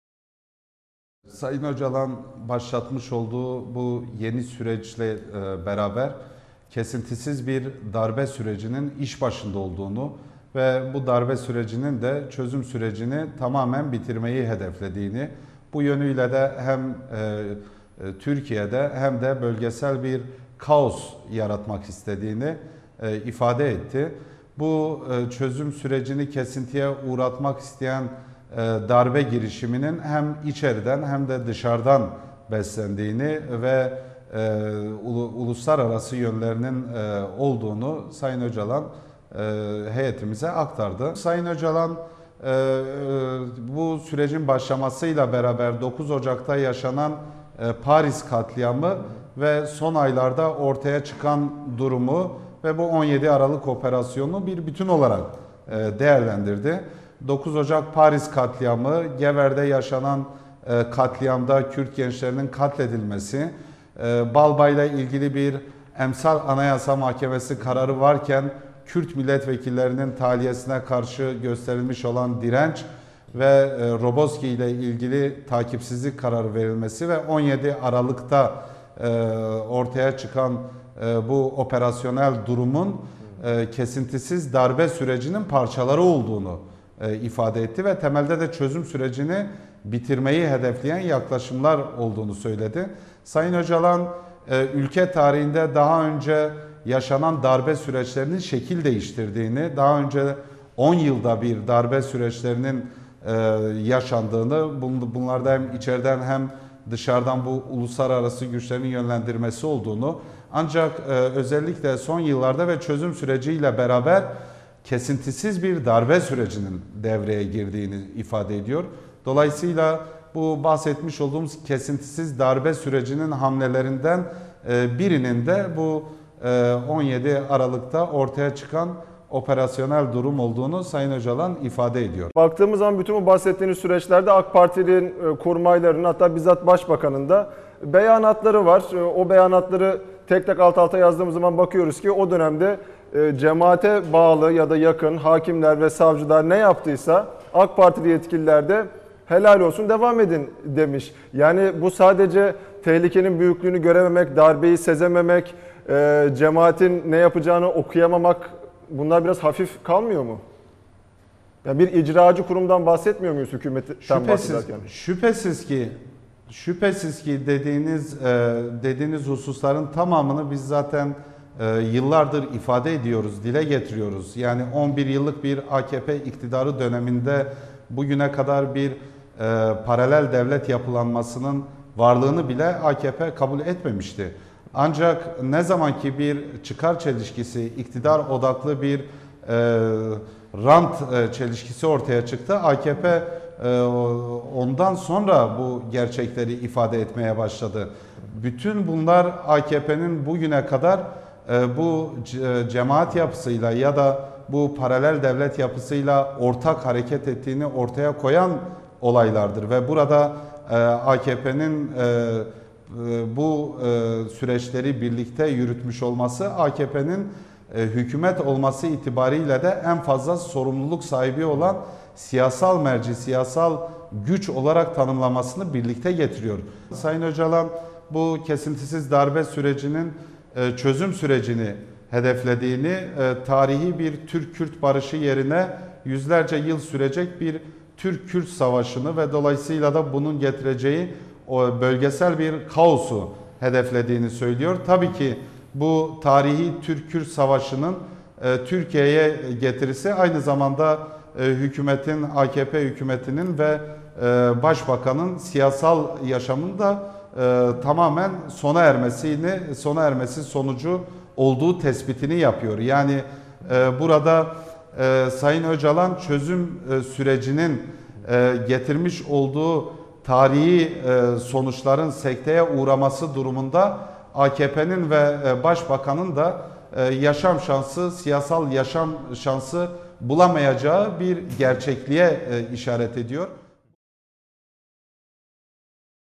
Öcalan'a göre 17 Aralık darbe
Yolsuzluk ve rüşvet operasyonu ve Hükümet - Cemaat çatışmasına  ilişkin PKK lideri Abdullah Öcalan ne düşünüyor. BDP Grup Başkan Vekili İdris Baluken Mustafa Kuleli'nin sorularını yanıtladı